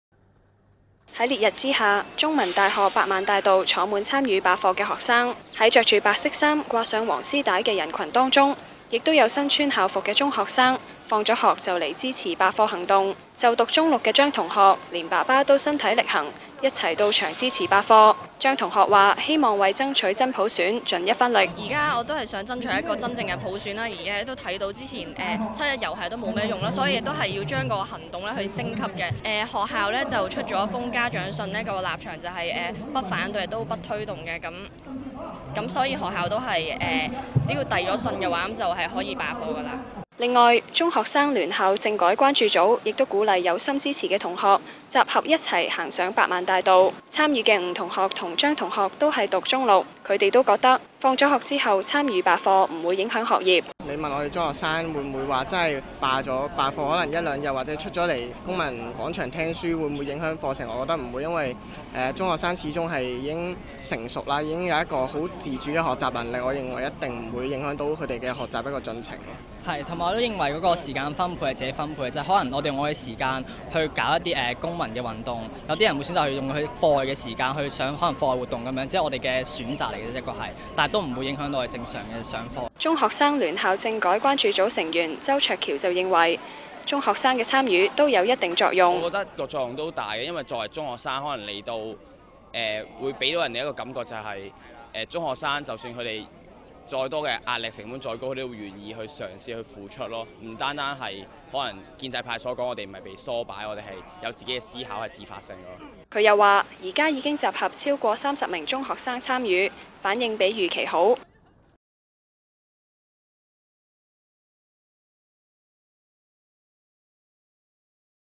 有中學生放學後到中大百萬大道參與罷課集會 (2014/09/22)
有中學生放學之後，到中大百萬大道參與罷課集會，部分有家長陪同出席。有中學生認為，自己有自主學習的能力，懂得分配時間，不擔心罷課會影響學業。